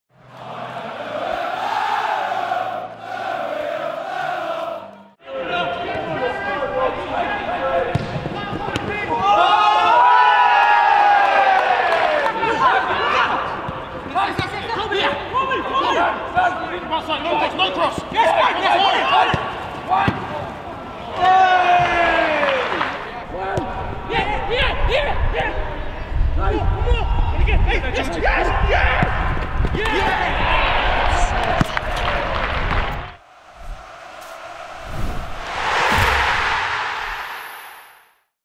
Notts County Reserves beat Hartlepool United 3-0
A youthful Notts County side beat Hartlepool United 3-0 in the FInal Third Development League to remain unbeaten for the 2015/16 season.